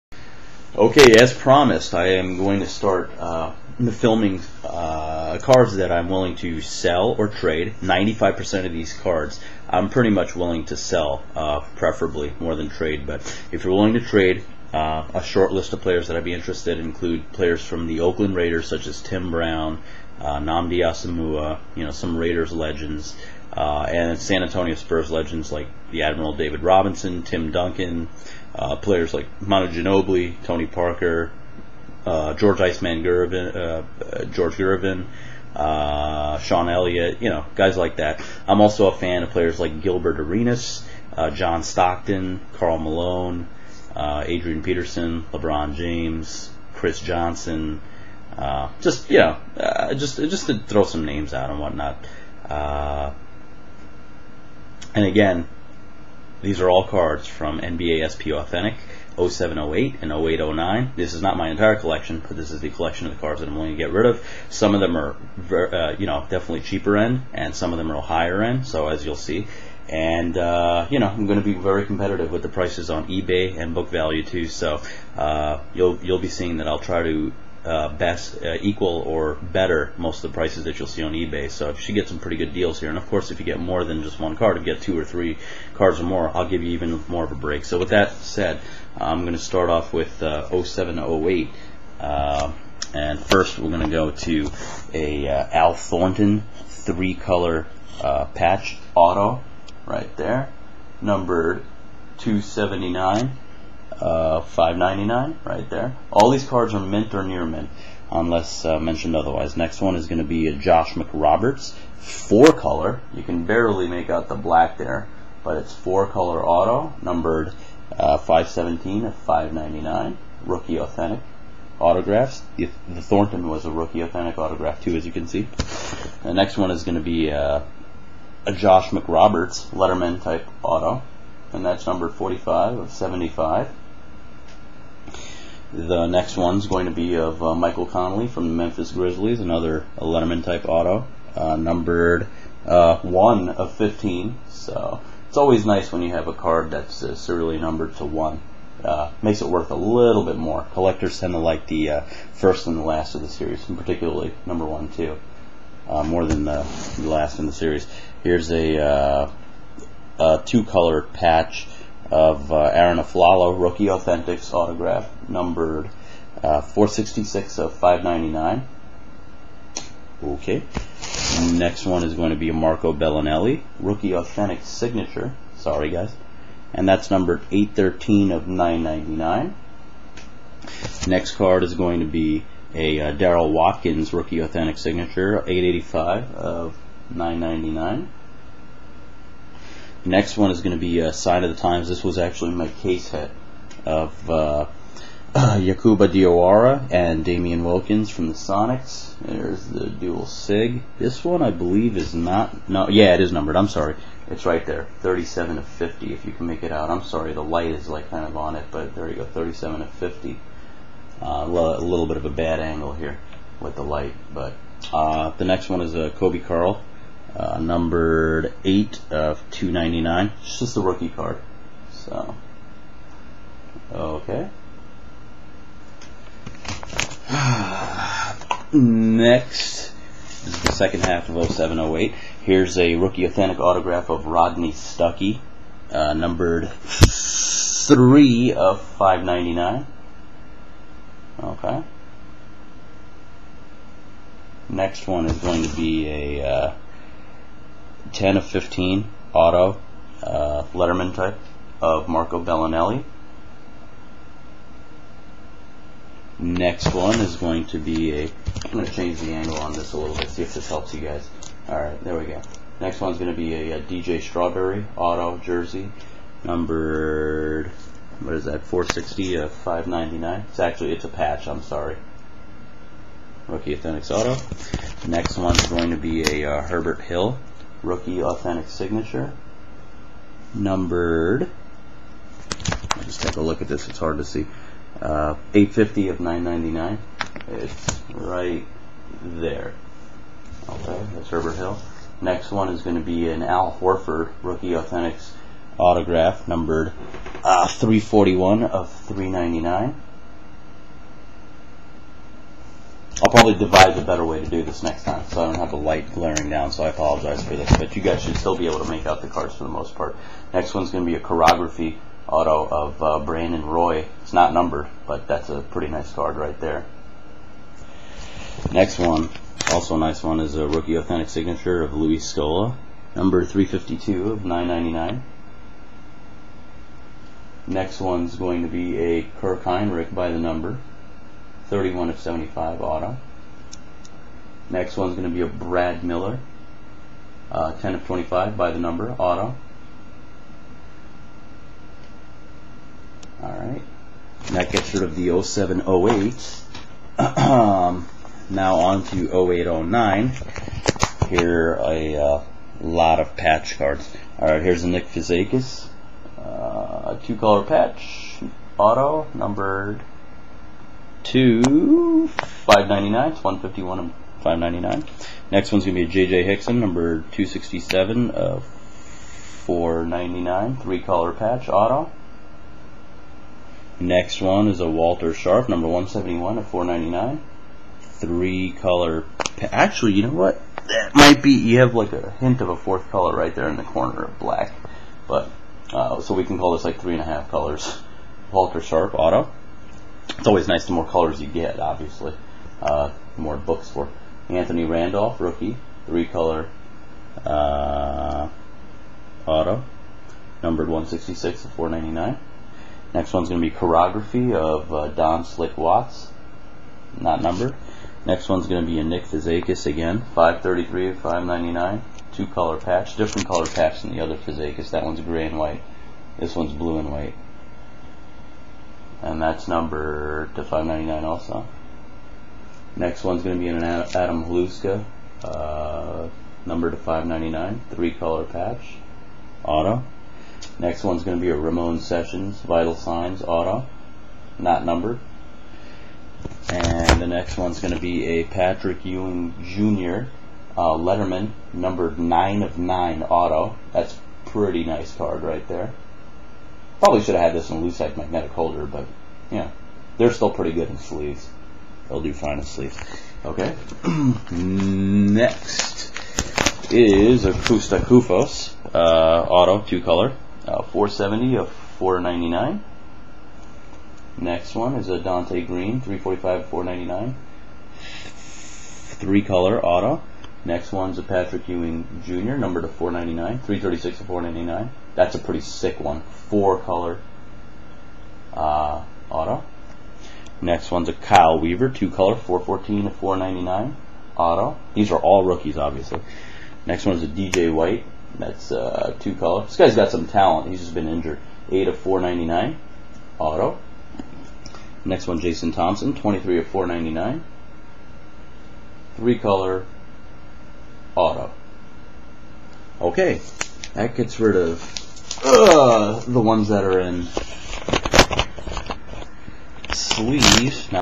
Az's UD SP Authentic 07/08/09 Basketball Cards for Sale/Trade!
spa authentic sale:

the following are #'d to /499:
kosta koufos auto jersey - 10
kyle weaver auto jersey- 17
d.j. white auto jersey- 15
donte green auto patch (3 color)- 10
patrick ewing jr auto patch(4 color) - 35
jason thompson auto patch(3 color) - 25
walter sharpe auto patch(3 color) - 12
anthony randolph auto patch(3 color) - 35
j.j. hickson auto patch(3 color)- 35
nick fazekas auto patch(grey/white) - 10
nick fazekas auto patch(blue/white) - 10
adam haluska auto patch(4 color) - 17

not numbered:
don watts chirography auto - 12
brandon roy chriography auto- 40
vlade divac chirography auto - 20
ramon sessions vital signs auto - 10
rudy fernandez vital signs auto - 17
al horford vital signs auto - 10

patrick ewing jr recruiting class auto #'d to 9 - 22
al horford recruiting class auto #'d to 25 - 50
don "slick" watts varsity letters auto #'d to 3 - 60
sasha vujacic varsity letters auto #'d to 7 - 30
marreese speights recruiting class auto #'d to 5 - 50

SOLD!!! - rodney stuckey rc auto #'d to 599 - 25
marco belinelli recruiting class auto #'d to 15 - 35
d.j. strawberry rc auto #'d to 59 - 15
al horford rc auto #'d to 399 - 60
herbert hill rookie auto #'d to 999 - 10
luis scola rc auto #'d to 999 - 15

kirk hinrich by the number auto #'d to 75 - 40
brad miller by the number auto #'d to 25 - 20
al thornton rc 3 color patch auto #'d to 599 - 45
josh mcroberts 4 color patch auto #'d to 599 - 15

josh mcroberts recruiting class auto #'d to 75 - 10
michael conley recruiting class auto #'d to 15 - 200
arron afflalo rookie 2 color patch auto #'d to 599 - 20
marco belinelli rookie auto #'d to 999 - 7
darryl watkins rookie auto #'d to 999 - 5
coby karl rookie #'d to 299 - 3
sign of the times dual auto damien wilkins & yakhouba diawara #'d to 50 - 20